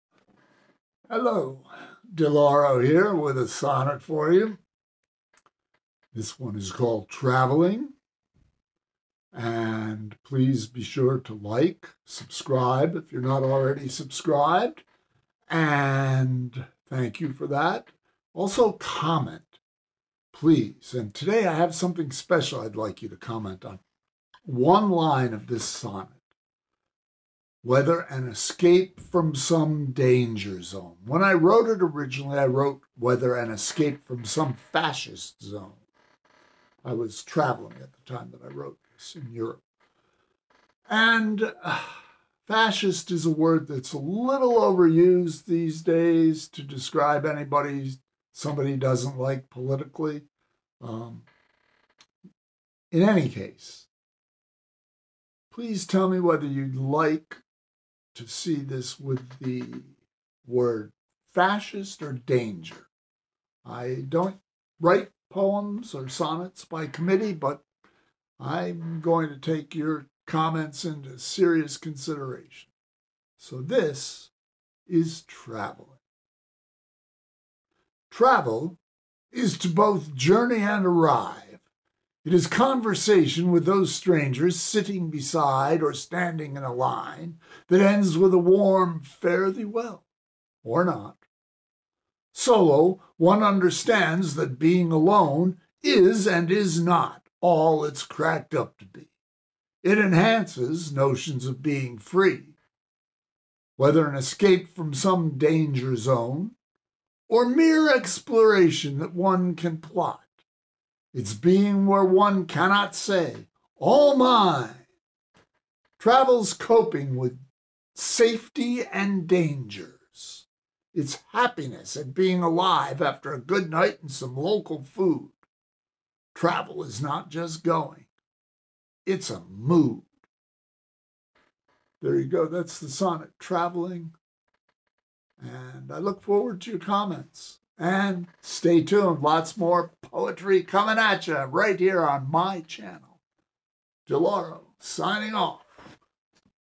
TRAVELLING - a sonnet
TRAVELLING

Travel is both to journey and arrive.
It is conversation with those strangers
Sitting beside or standing in a line
That ends with a warm fare-thee-well, or not.
Solo one understands being alone
Is and is not all it’s cracked up to be.
It enhances notions of being free.

Whether an escape from some fascist zone
Or mere exploration that one can plot.
It’s being where one cannot say, “All mine.”
Travel’s coping with safety and dangers.
It is happiness at being alive.
After a good night and some local food
Travel is not just going. It’s a mood.
                               - DiLauro